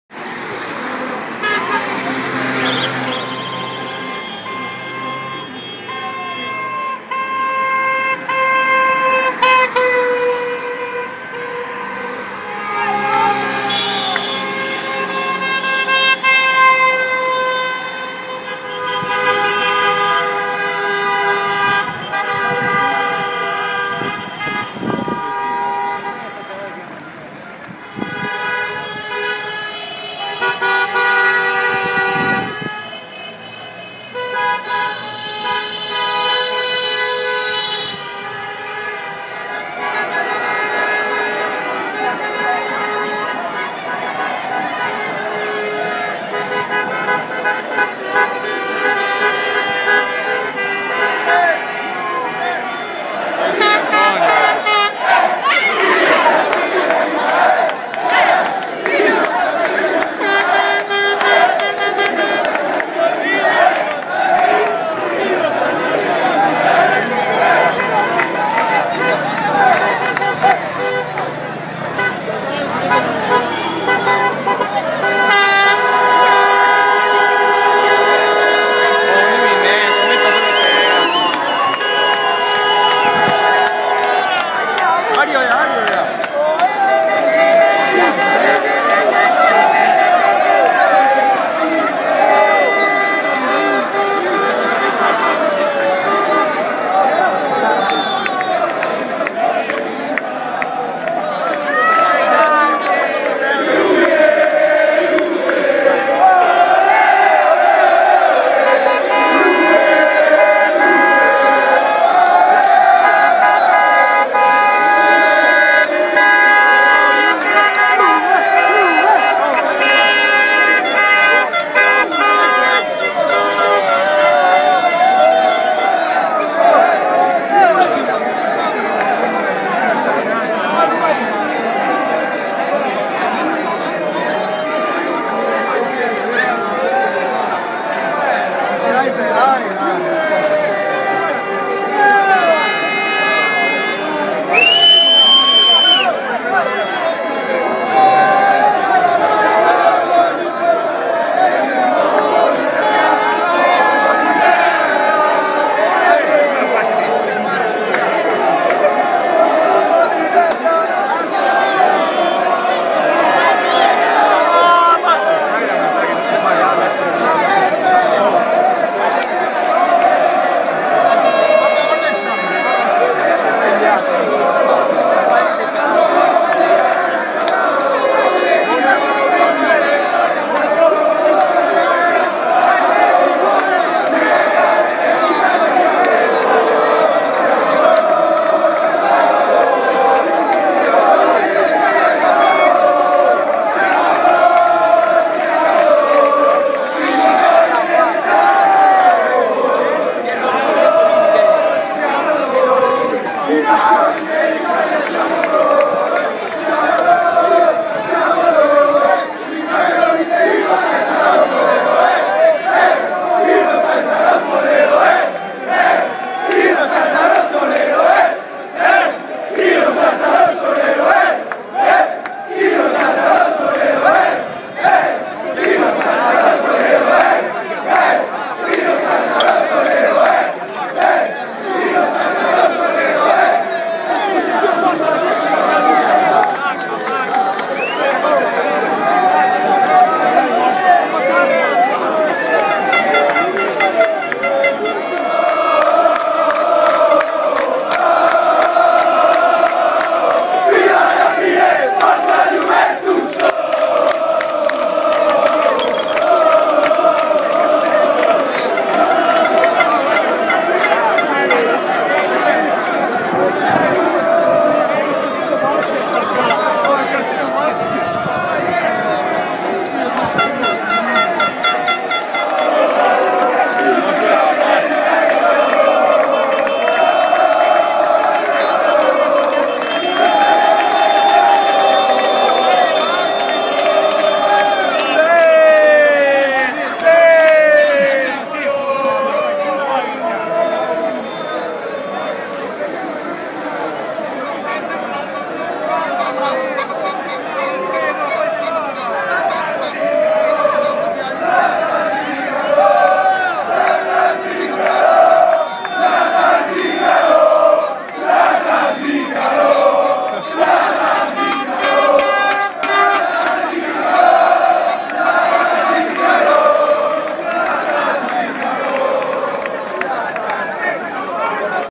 Sottomarina - I festeggiamenti per il 30° Scudetto della Juventus
I festeggiamenti per il 30° Scudetto della Juventus sul lungomare di Sottomarina (VE).